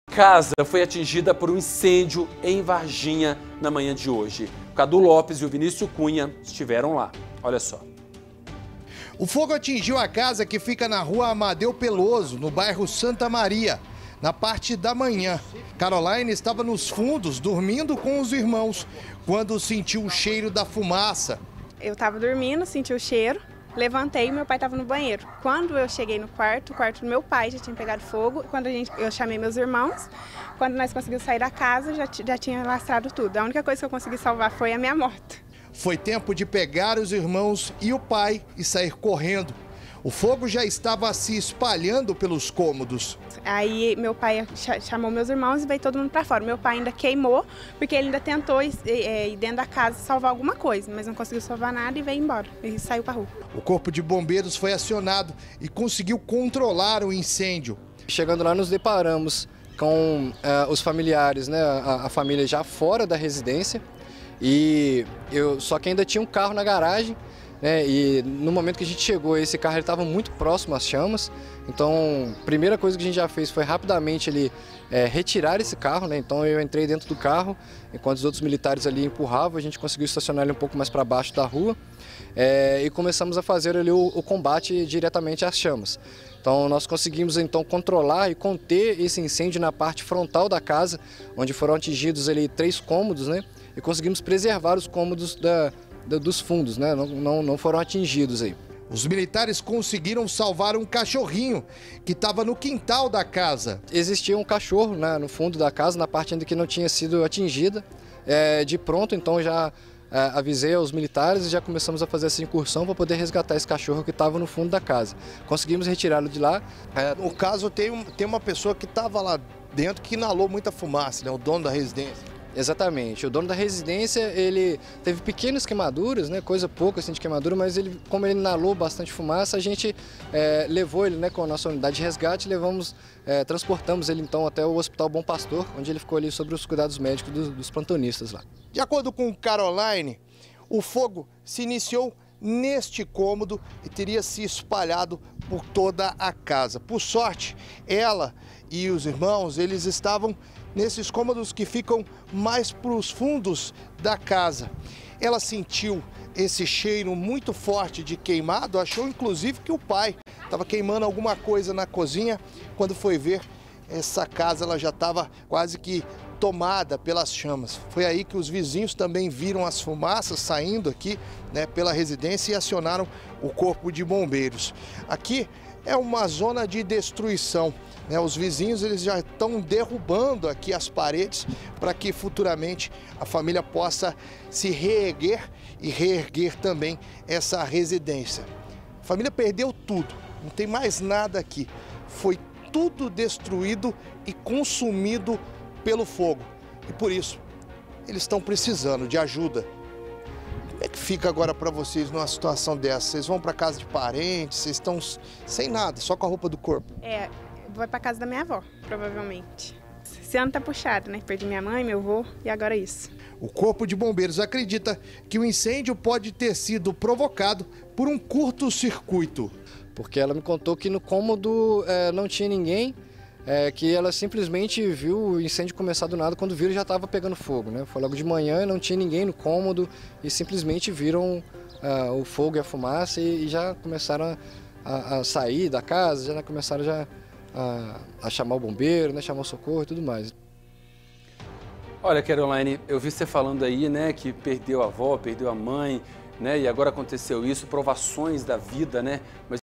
Incêndio destruiu casa em Varginha
VARGINHA | Incêndio destruiu casa em Varginha. Veja que a família perdeu tudo. Acompanhe na reportagem.